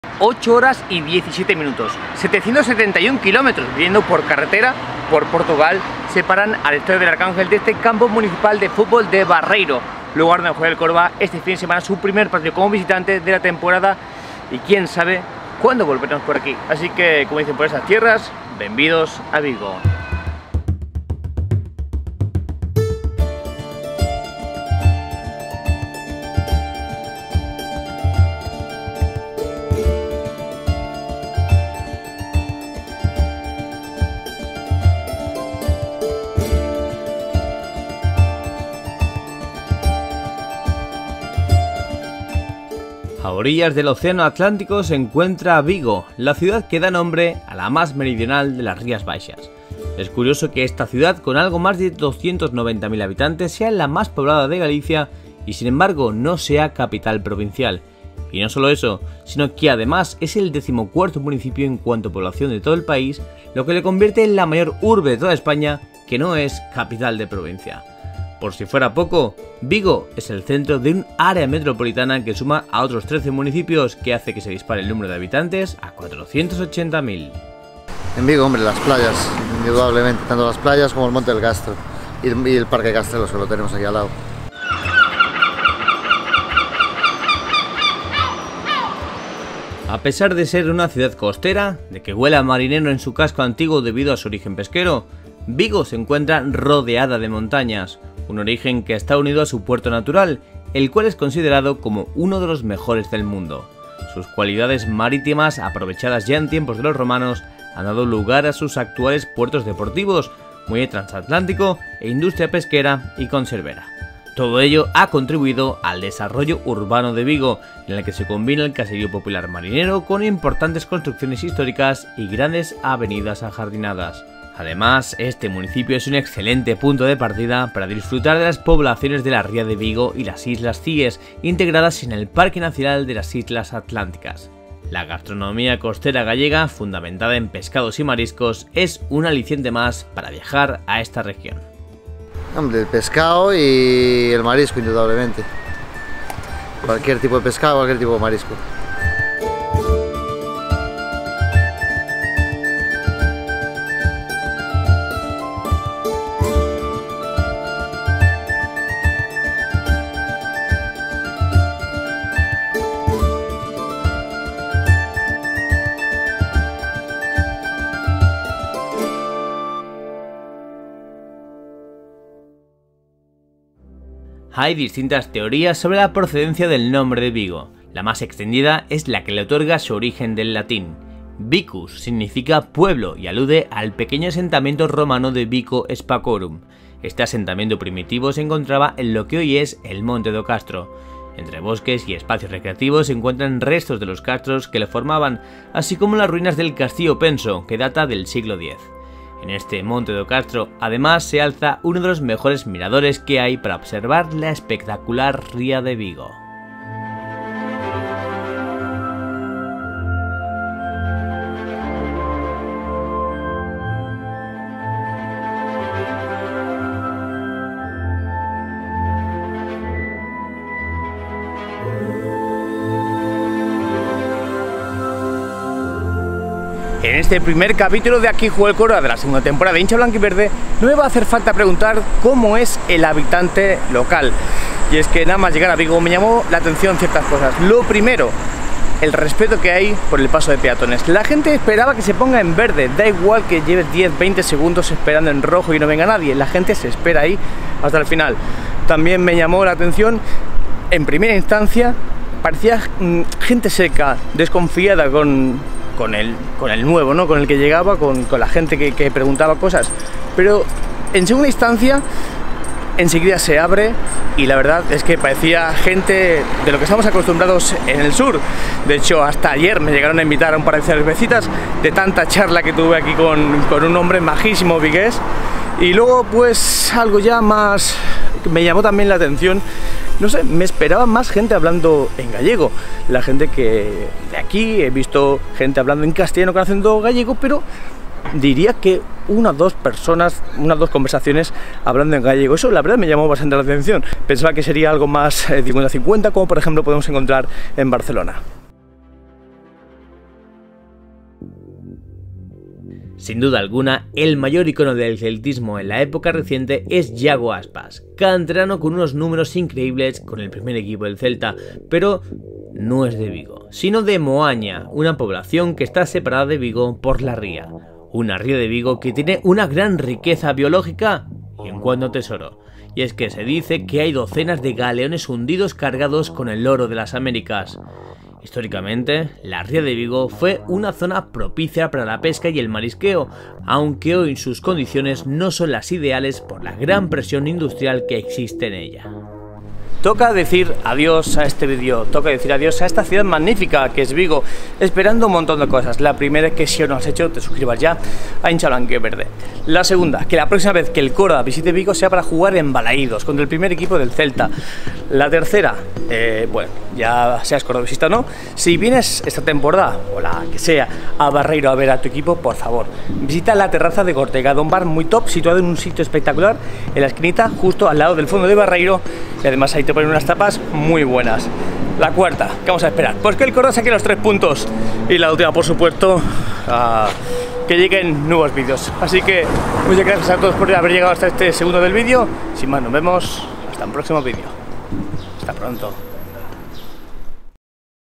VIGO | La ciudad más meridional de las Rías Baixas
A orillas del Océano Atlántico, se encuentra Vigo, la ciudad que da nombre a la más meridional de las Rías Baixas.

Con algo más de 290.000 habitantes, es la más poblada de Galicia y, sin embargo, no es capital provincial. Y no sólo eso, sino que, además, es el decimocuarto municipio en cuanto a población de todo el país, lo que le convierte en la mayor urbe de toda España que no es capital de su provincia.

A pesar de ser una ciudad costera, de que huela a marinero en su casco antiguo debido a su origen pesquero, Vigo se encuentra rodeada de montañas. Un origen que está unido a su puerto natural, el cual es considerado como uno de los mejores del mundo.